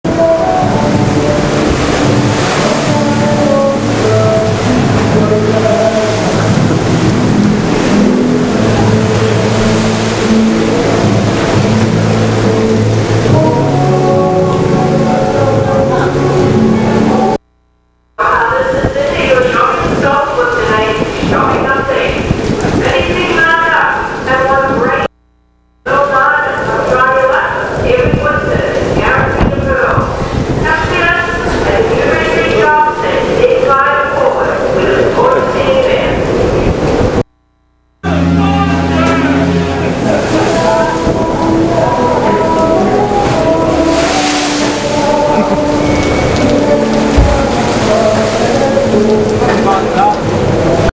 Rough Seas
My Island hopping on Norwegians Pride of America cruise ship in Hawaii was a rough one. Seas got so rough (broke 3 bow lines) we had to leave about 300 passengers and crew on land in Maui until the next morning.